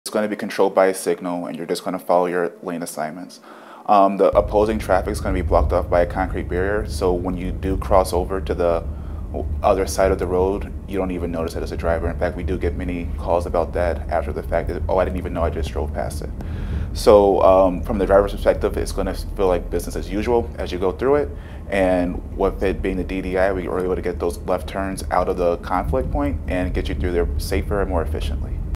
How to drive through a diverging diamond interchange
Driving on the left-hand side of the road? In the U.S.? A new traffic pattern is coming to more interchanges. Wisconsin traffic engineer Mason Simmons explains how to drive through a diverging diamond.